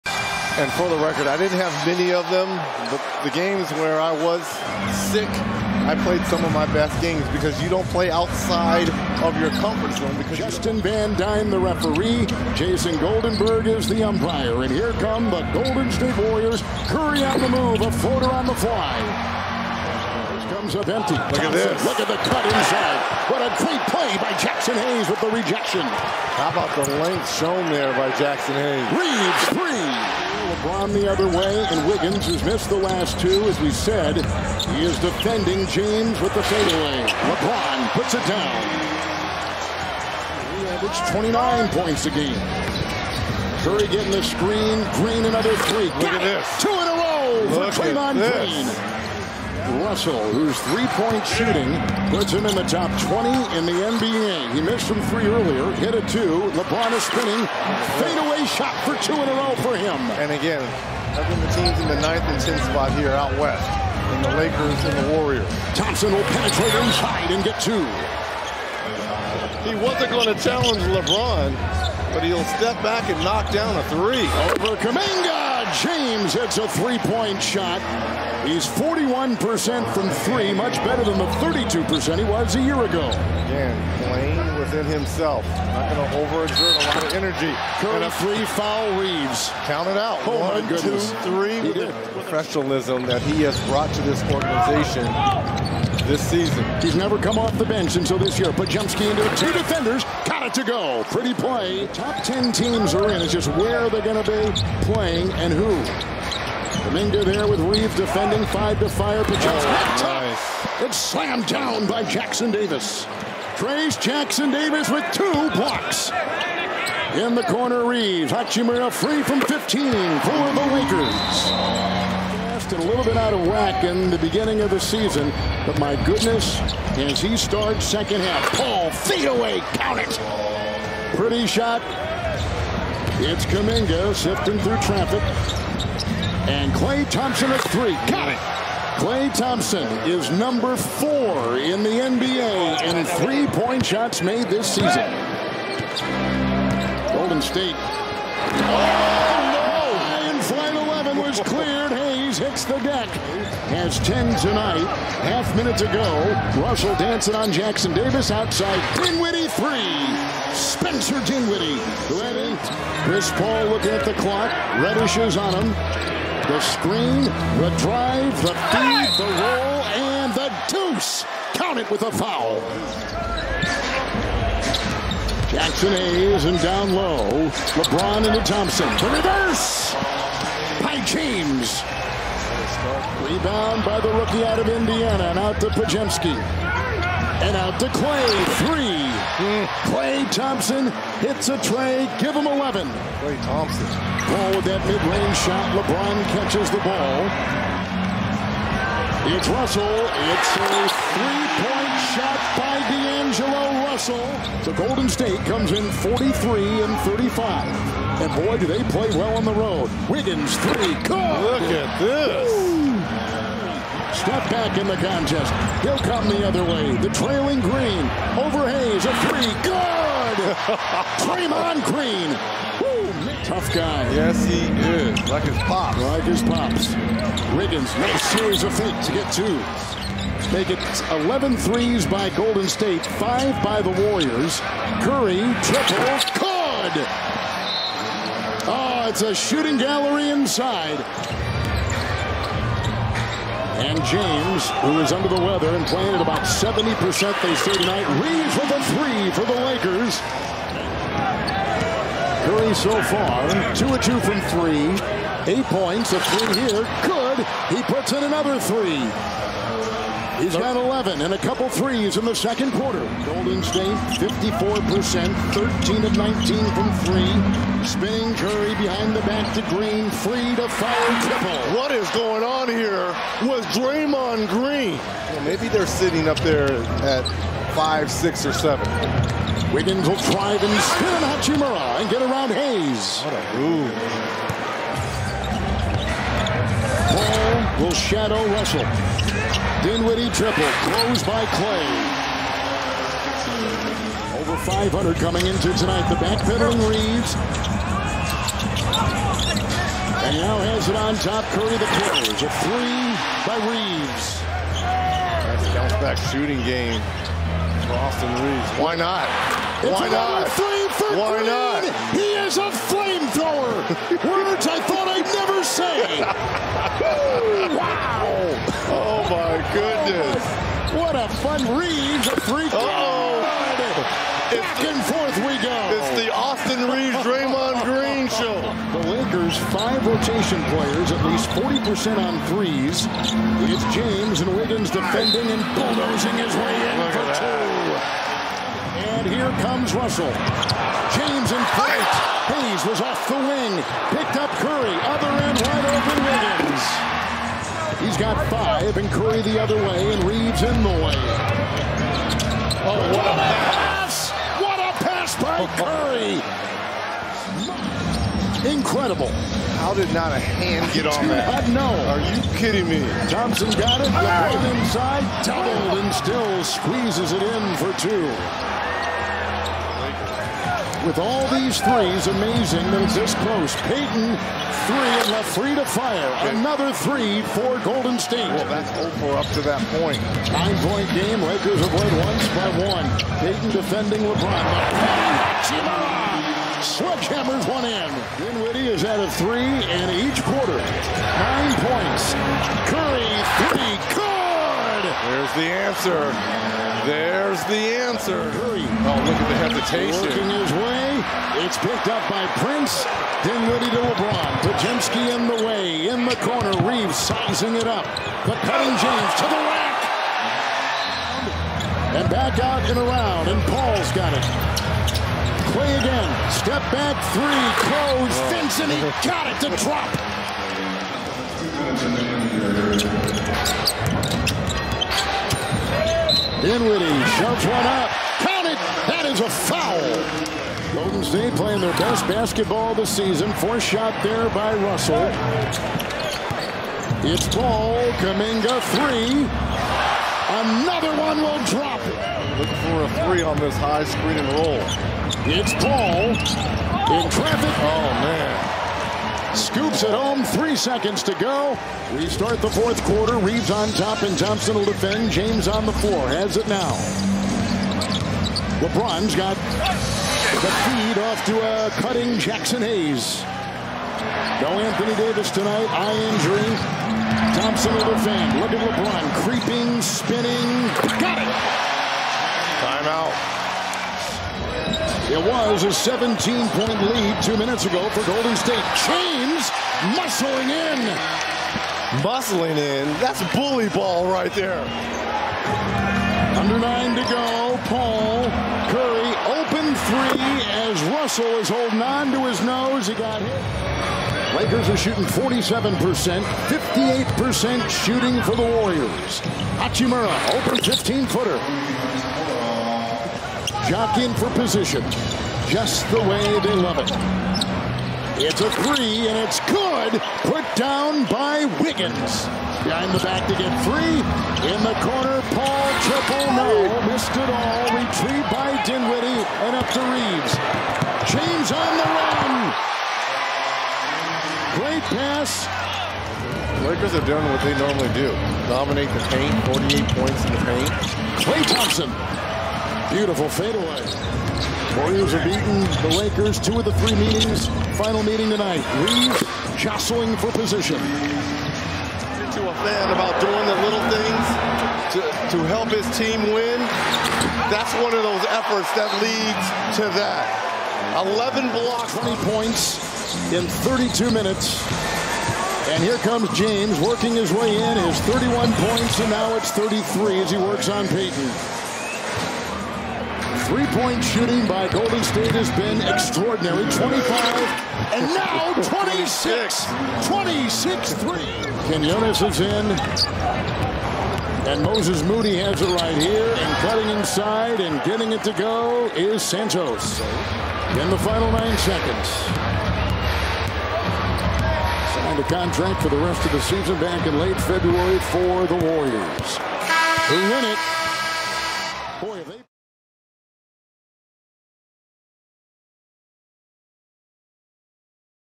WARRIORS at LAKERS | FULL GAME HIGHLIGHTS | April 9, 2024
The Golden State Warriors defeated the Los Angeles Lakers 134-120. Klay Thompson recorded 27 points, 5 3PM, and 3 assists for the Warriors, with Stephen Curry adding 23 points (6-6 3PM), 8 assists, and 7 rebounds in the victory. LeBron James tallied 33 points, 11 assists, and 7 rebounds for the Lakers. The Warriors improve to 44-35, while the Lakers fall to 45-35.

With their performance tonight (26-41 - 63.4%), the Warriors become the first team in NBA history to make 25+ 3PT FGs AND shoot 60% or better on 3PT attempts.
Previously, the highest mark for a team was 59.5% (25-42) from the Memphis Grizzlies on 3/24/2023